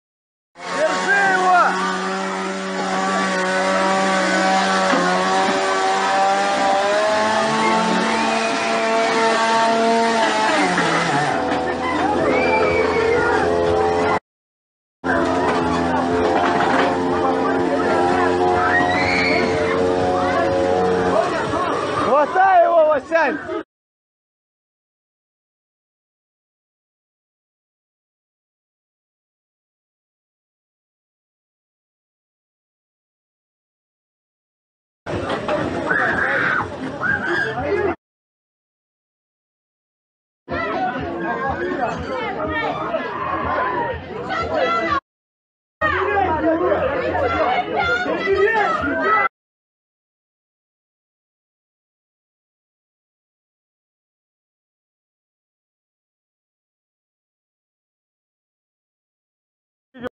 Комсомольчанин устроил в Приморье "резню" бензопилой
30-летний командировочный из Комсомольска-на-Амуре пришел в кафе Находки с работающей бензопилой